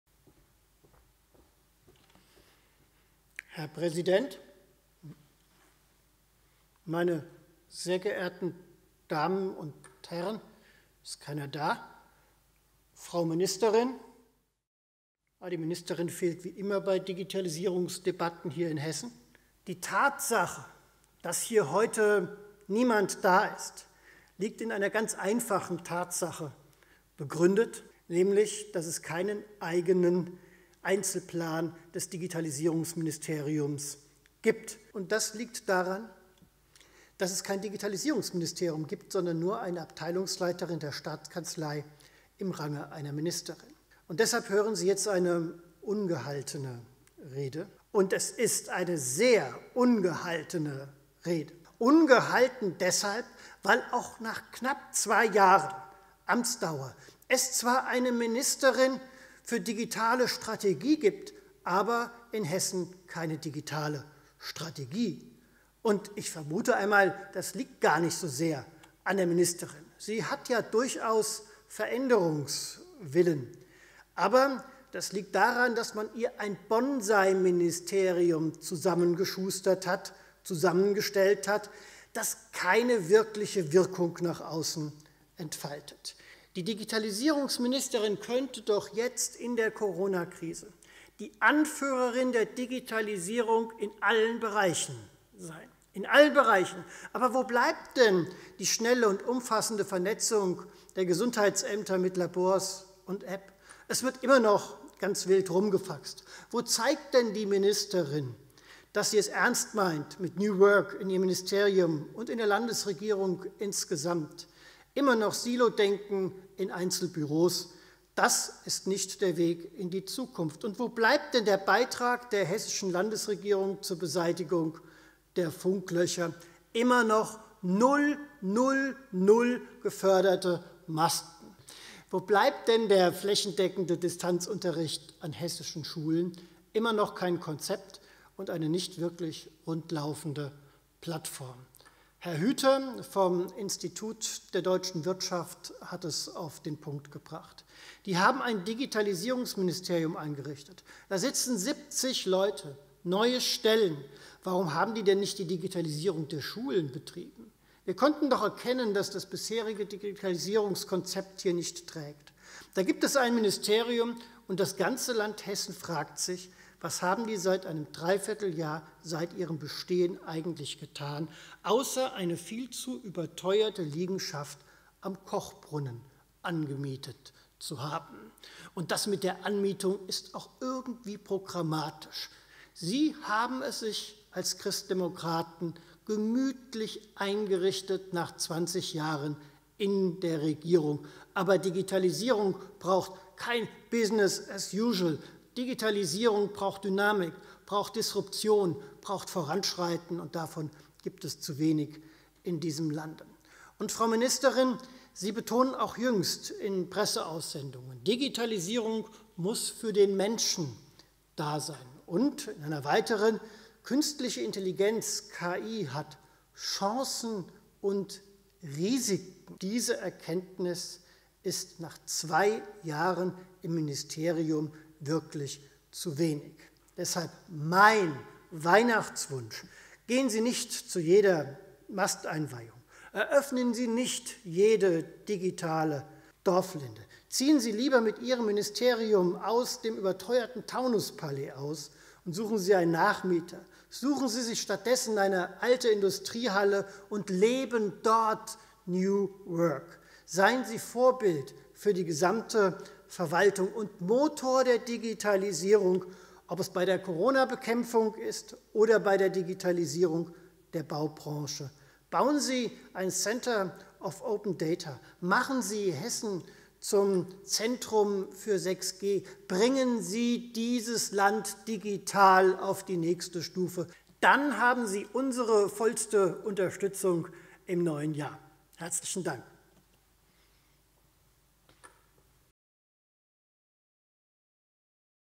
Eine ungehaltene Rede zur Digitalisierung
Wir präsentieren: Oliver Stirböcks „ungehaltene Rede“ zur Digitalisierung in Hessen. 💥
Natürlich digital. Denn leider hatte unser digitalpolitischer Sprecher bei den Haushaltsberatungen in der vergangenen Sitzungswoche keine Gelegenheit, über seinen Fachbereich zu sprechen.
Wieso das? Ganz einfach: Hessen hat zwar eine Ministerin für Digitalisierung – aber kein Digitalisierungsministerium.🤷‍♂️ Und somit gibt es auch keinen Einzelplan, der im Rahmen des Haushalts 2021 hätte beraten werden können. Mehr muss man zum Stellenwert der Digitalisierung in Hessen eigentlich gar nicht wissen! Dazu zu sagen, haben wir allerdings eine Menge… 📣